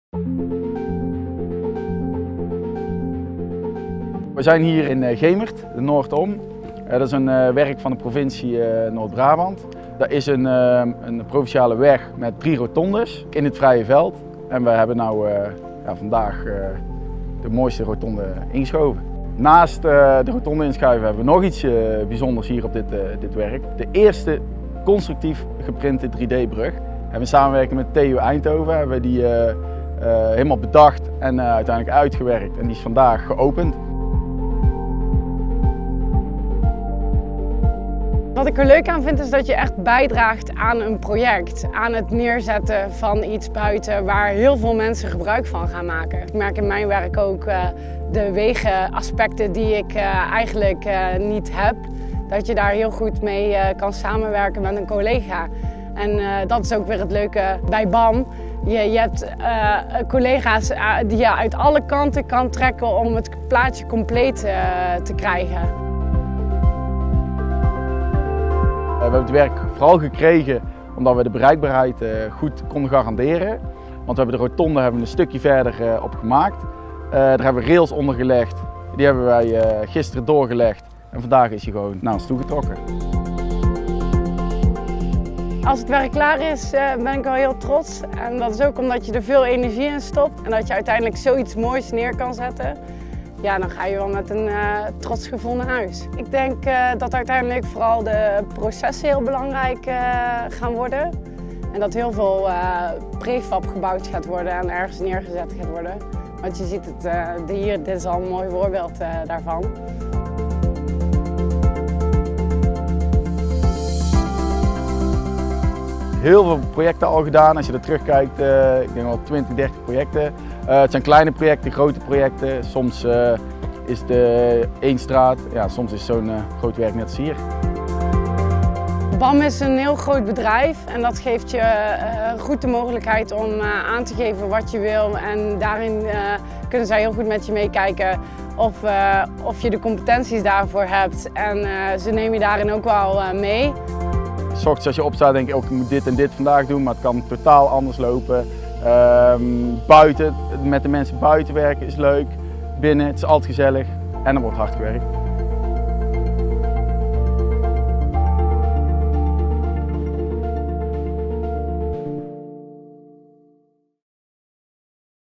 Werken bij BAM Infra als Werkvoorbereider
Als werkvoorbereider bij BAM Infra zet je jouw talenten in om innovatieve projecten te realiseren. Denk hierbij aan ’s werelds grootste fietsenstalling, de eerste geschoven rotonde in Nederland en de eerste 3D geprinte fietsbrug ter wereld. Nieuwsgierig naar meer? Neem contact op met onze recruiter Sanne van Vliet:
Of bekijk hier de vacatures Welkom bij BAM Infra!